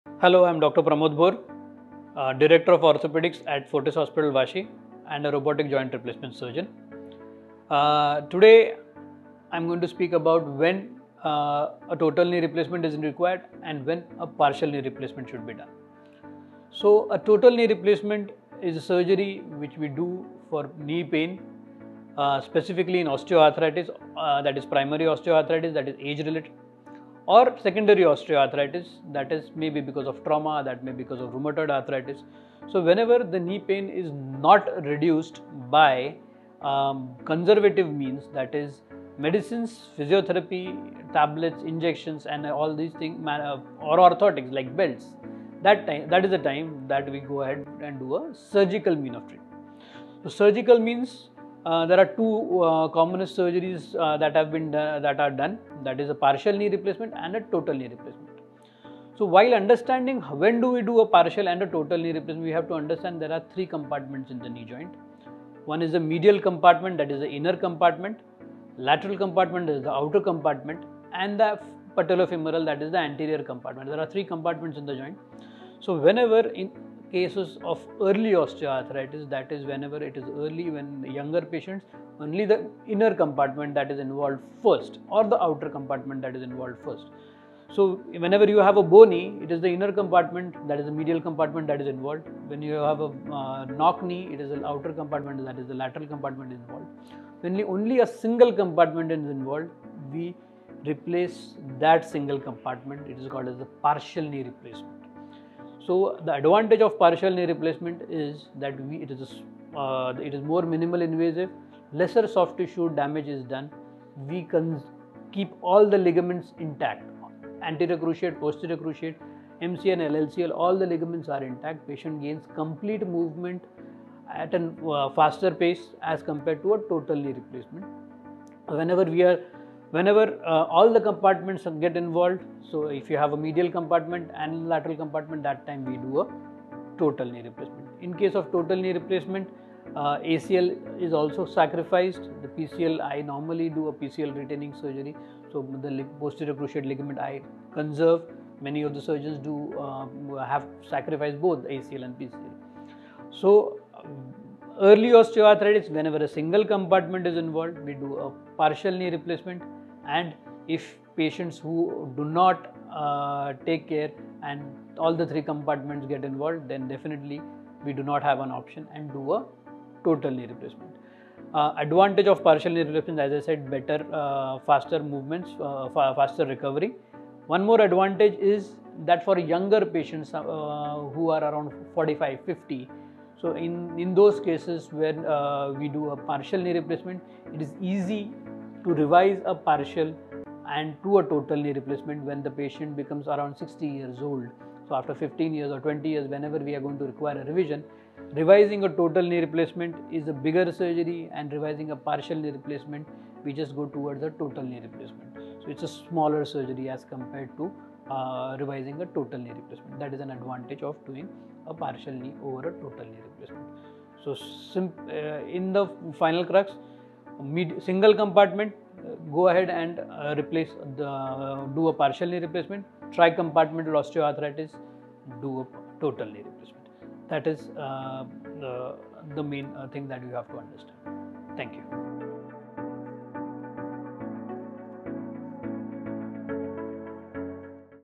When Is Total Partial Knee Replacement Required ? | Dr Pramod Bhor | VAS
In this enlightening video, Dr. Pramod Bhor, Director of Orthopaedics and Joint Replacement Surgeon at Fortis Hiranandani Hospital delves into the considerations for total or partial knee replacement in the context of knee osteoarthritis. When conservative treatments like medication and physical therapy fall short, surgical options become pivotal. Dr. Bhor outlines that total knee replacement is recommended for cases where all three knee compartments are affected. In comparison, partial knee replacement is a viable alternative for one or two compartments, aiming to preserve natural joint tissue.
Share this insightful content among your loved ones for informed health decisions.

इस ज्ञानवर्धक वीडियो में, फोर्टिस हीरानंदानी अस्पताल में ऑर्थोपेडिक्स के निदेशक और संयुक्त प्रतिस्थापन सर्जन डॉ. प्रमोद भोर घुटने के पुराने ऑस्टियोआर्थराइटिस के संदर्भ में पूर्ण या आंशिक घुटने के प्रतिस्थापन के विचारों पर प्रकाश डालते हैं। जब दवा और भौतिक चिकित्सा जैसे रूढ़िवादी उपचार कम पड़ जाते हैं, तो सर्जिकल विकल्प महत्वपूर्ण हो जाते हैं। डॉ. भोर बताते हैं कि उन मामलों में संपूर्ण घुटना प्रतिस्थापन की सिफारिश की जाती है, जहां घुटने के तीनों हिस्से प्रभावित होते हैं। इसकी तुलना में, आंशिक घुटने का प्रतिस्थापन एक या दो डिब्बों के लिए एक व्यवहार्य विकल्प है, जिसका लक्ष्य प्राकृतिक संयुक्त ऊतक को संरक्षित करना है।
जानकारीपूर्ण स्वास्थ्य निर्णयों के लिए इस ज्ञानवर्धक सामग्री को अपने प्रियजनों के बीच साझा करें।

Chapters:
0:00 Intro
00:12 When Is Total Partial Knee Replacement Required ?
0:56 Common surgery for arthritis
01:22 Total knee replacement
2:53 Advantage of partial knee replacement
03:55 End